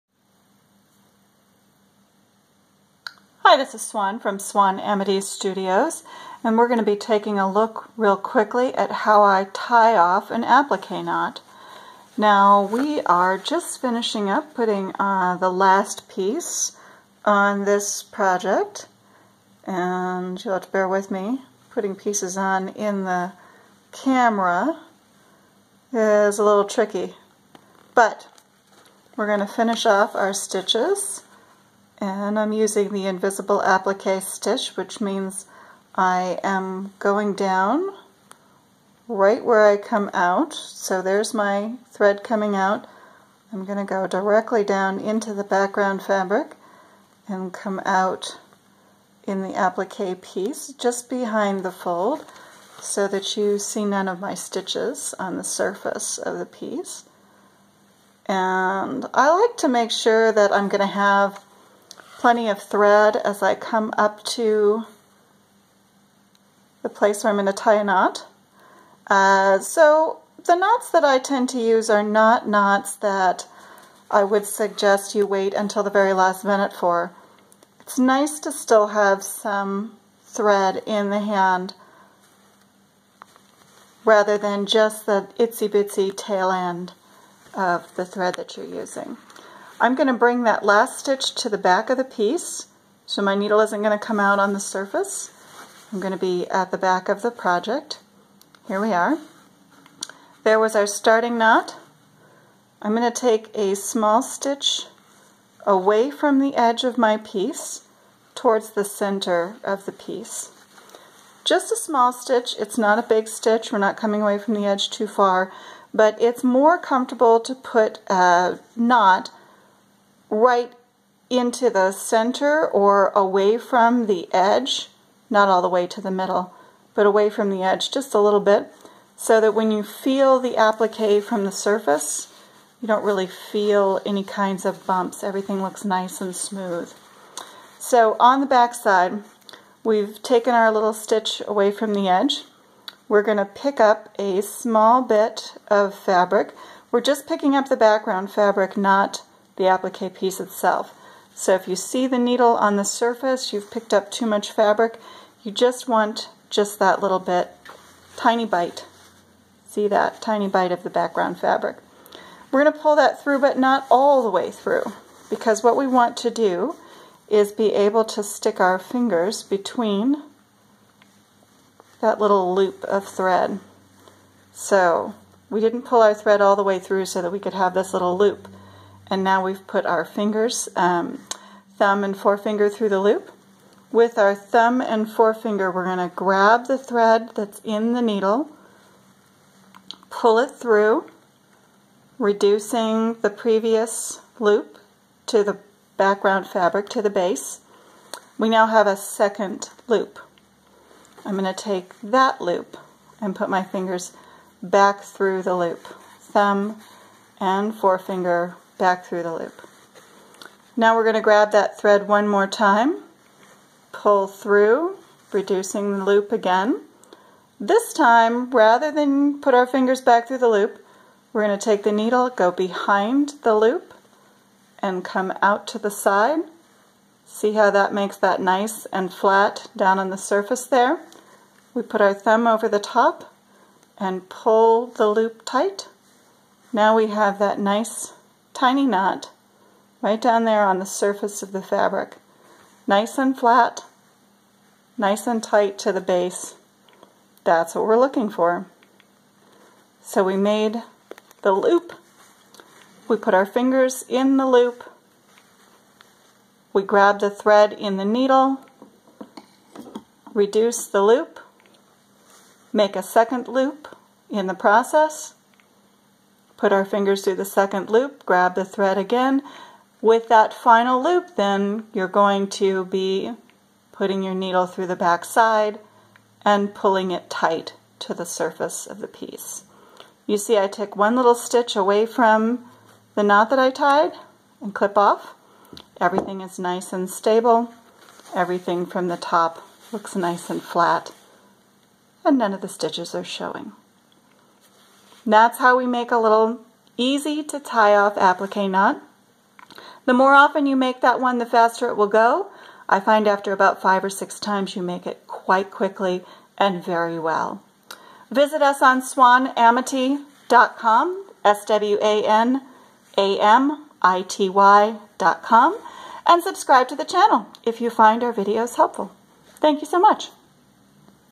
Making a finishing knot
How to tie off an applique knot smoothly and flat to the surface of the project.